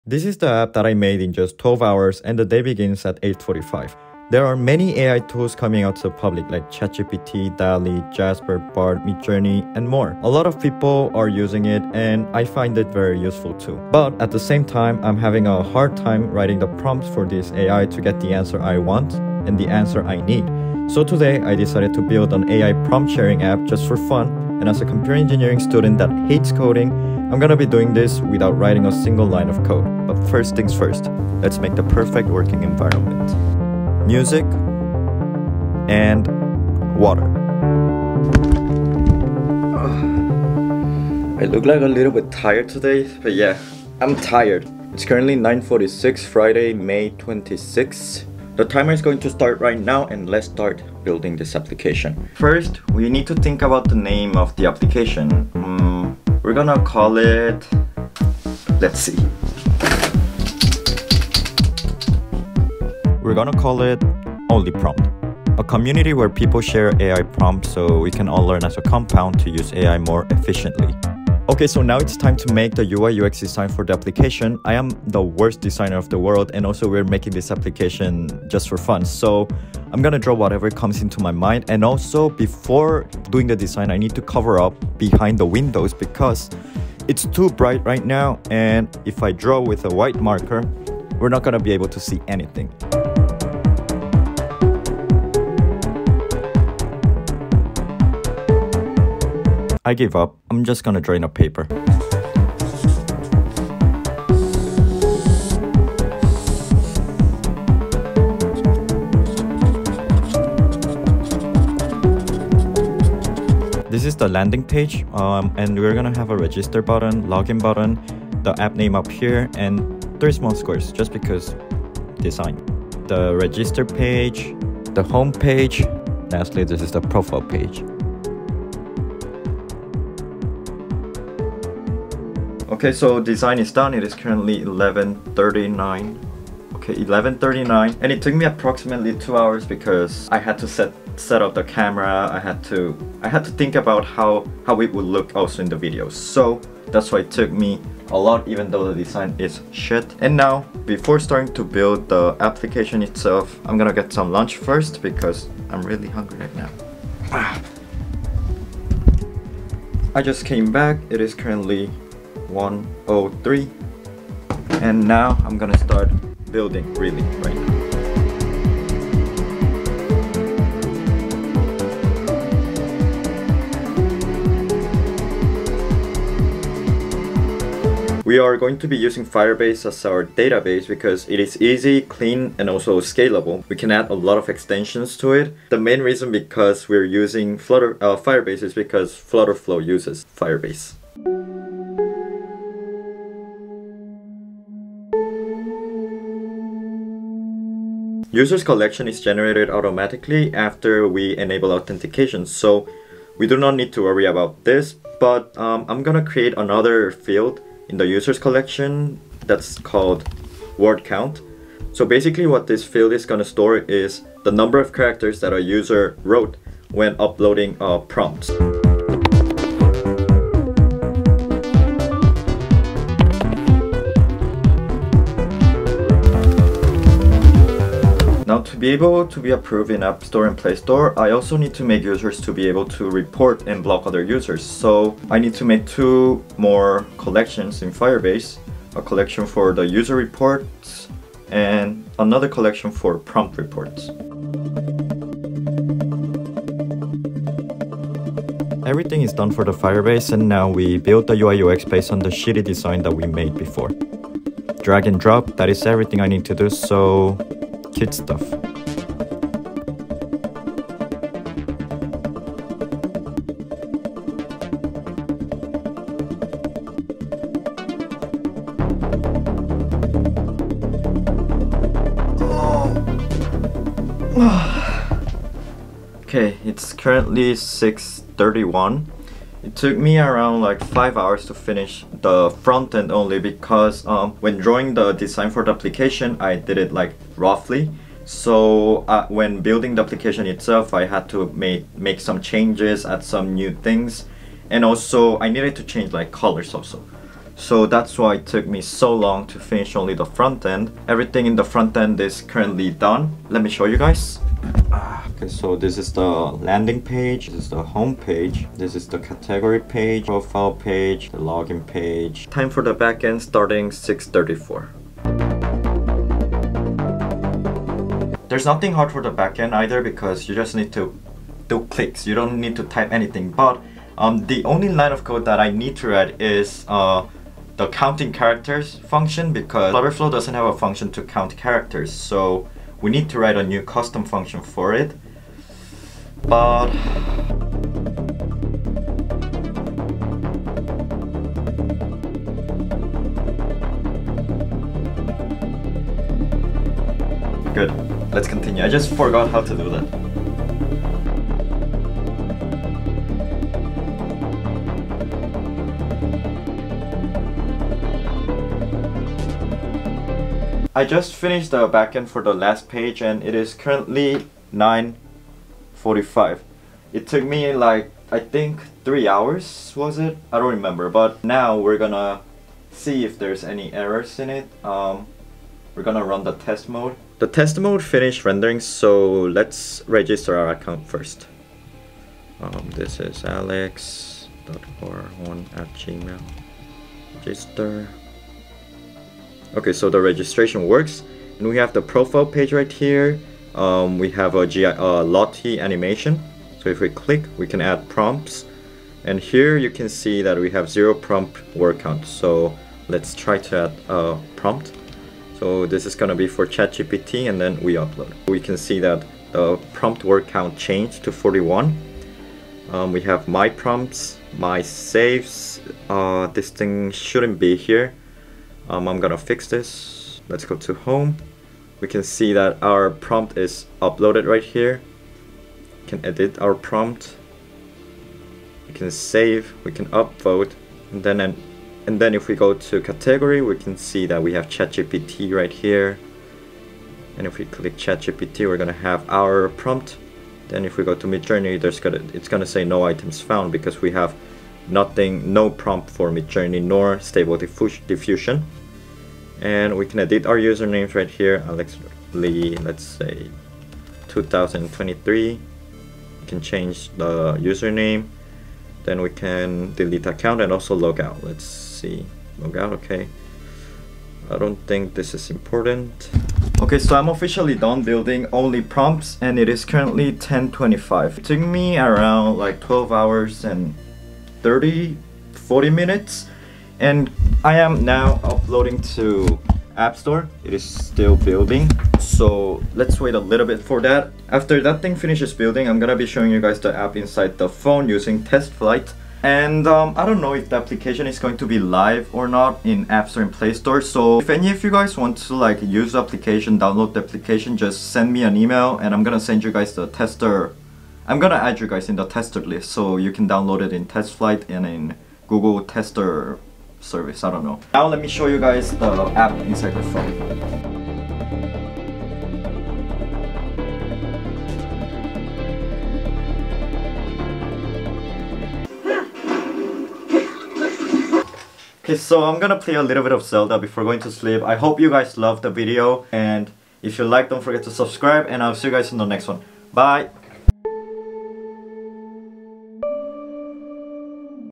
Building an App in just 12 Hours - by a computer engineering Student (NOT CLICKBAIT)
I started building the application at 9:46AM and ended at 10:25PM so it took me exactly 12 hours and 39 minutes to finish building the entire application.

I built this application without writing a single line of code because I am a computer engineering student that doesn't like coding 😅 That's why I use FlutterFlow, a no code app builder.